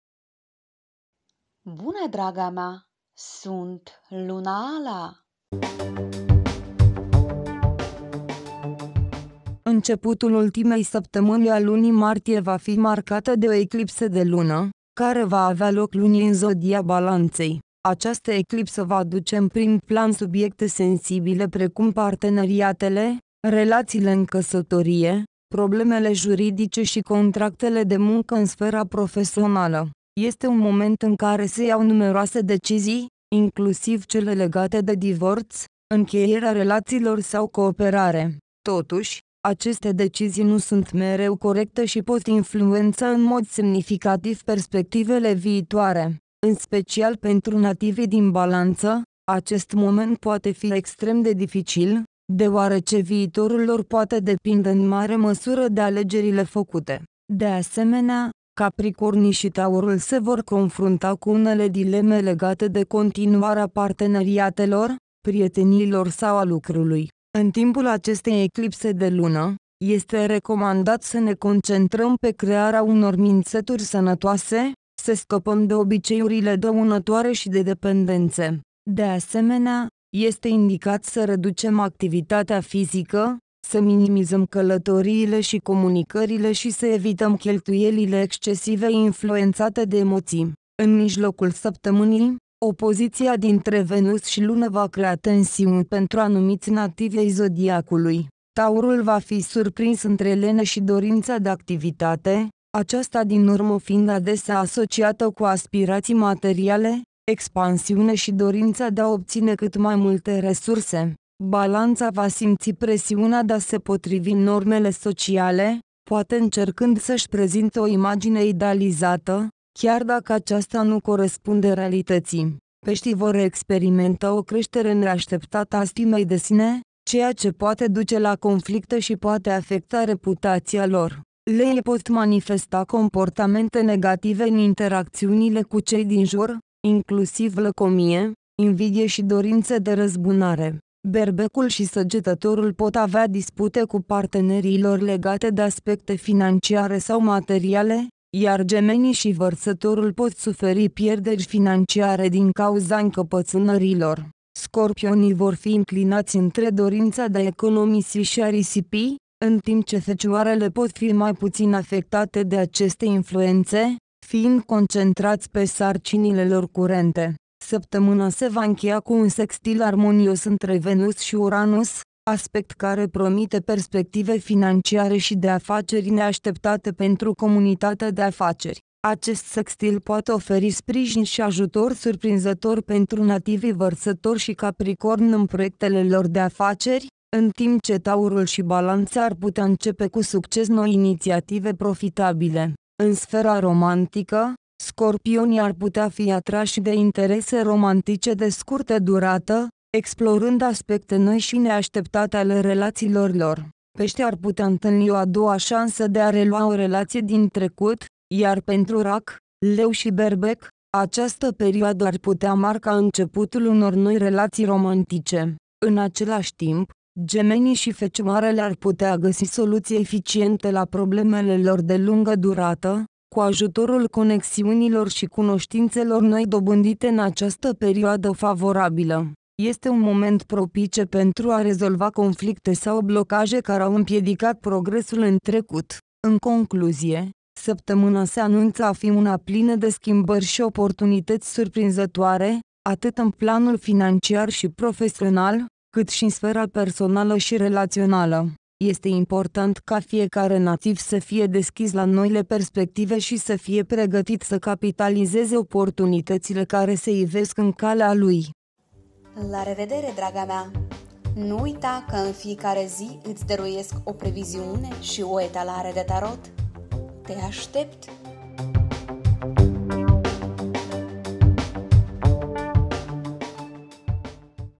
Horoscop saptamanal 25 la 31 Martie: cine o cerere în căsătorie și cine un mare bonus
Îți spun totul despre planurile Universului pentru următoarele zile. Interpretez mesajele Universului pentru următoarele zile.